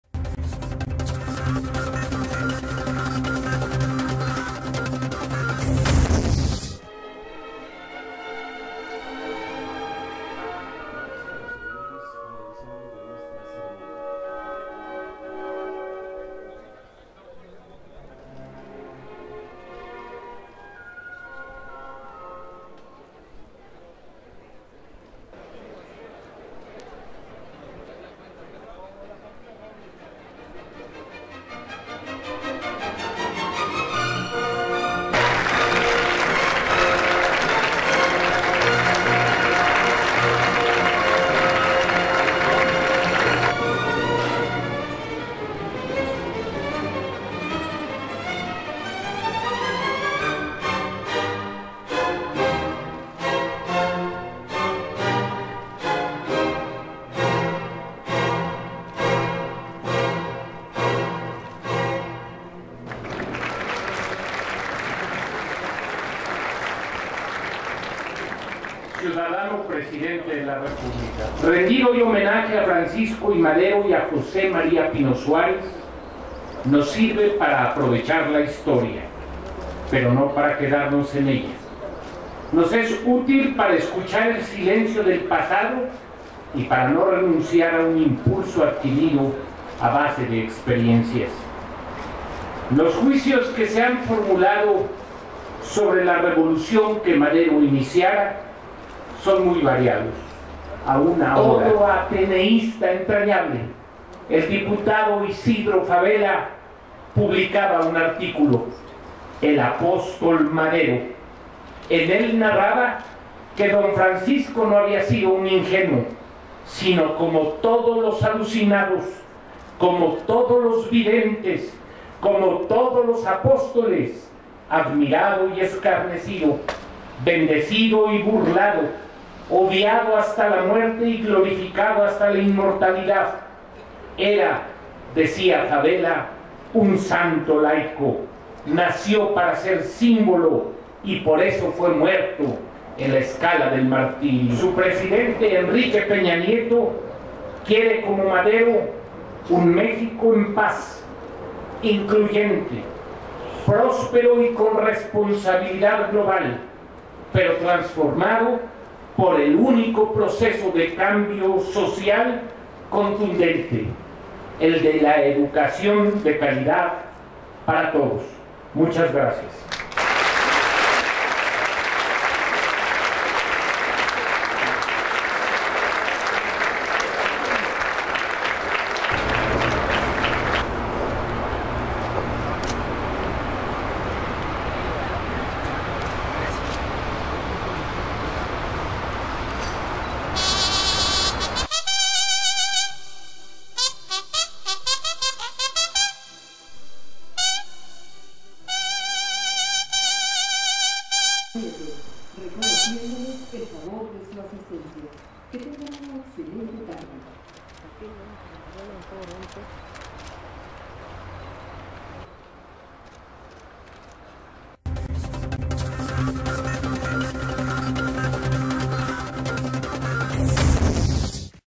Enrique Peña Nieto.Centenario Luctuoso de Francisco I. Madero
Francisco I. Madero,Centenario Luctuoso.
Enrique Peña Nieto Presidente de México
Emilio Chuayffet Chemor Secretaroi de Educación Pública